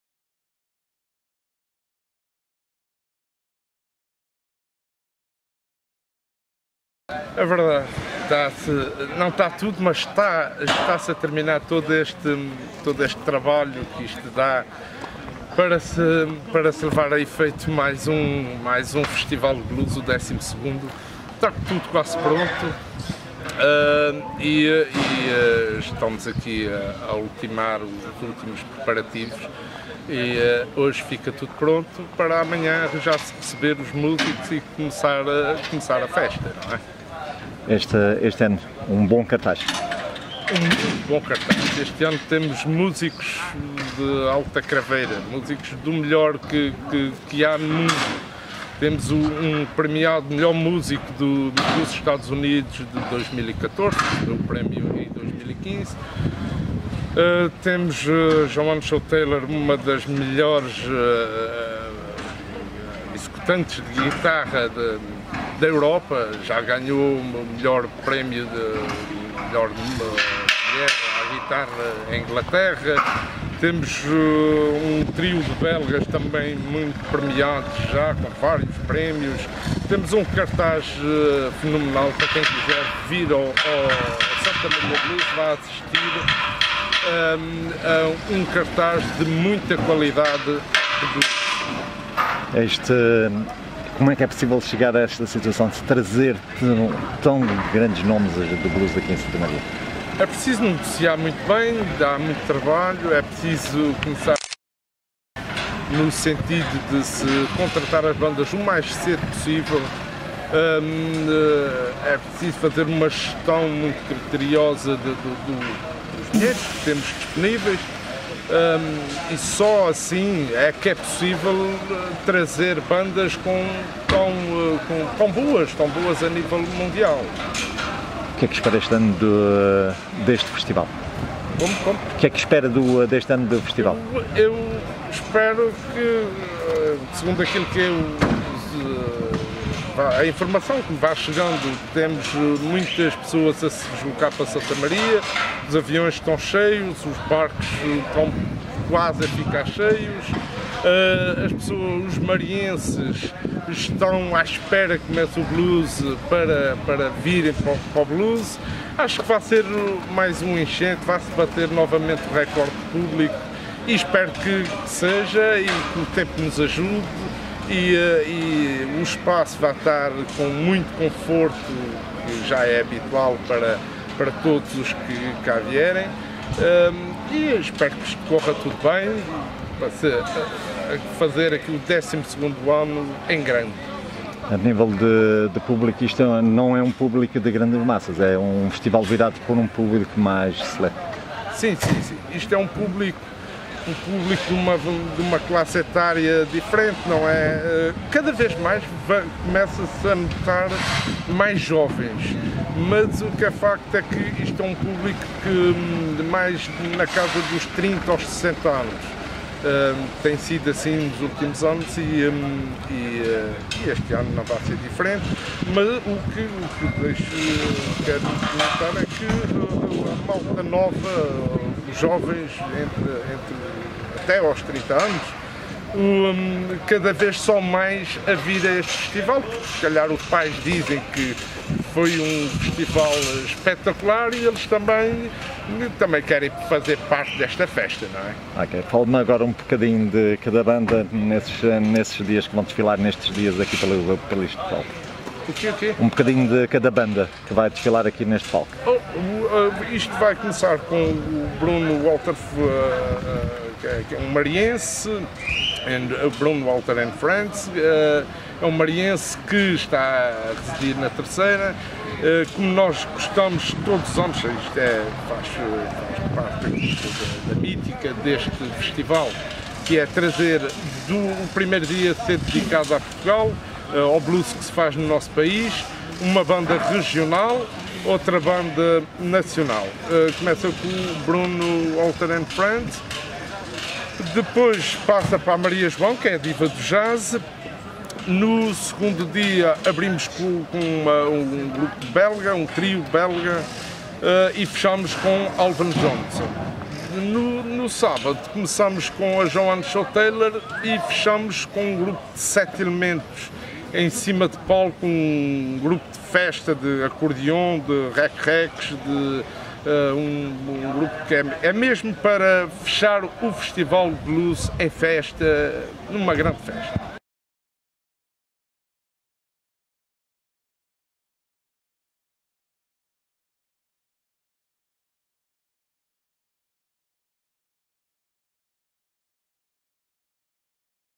entrevistapresidenteBlues205
Entrevista ao Presidente da Associação Escravos da Cadeinha António Monteiro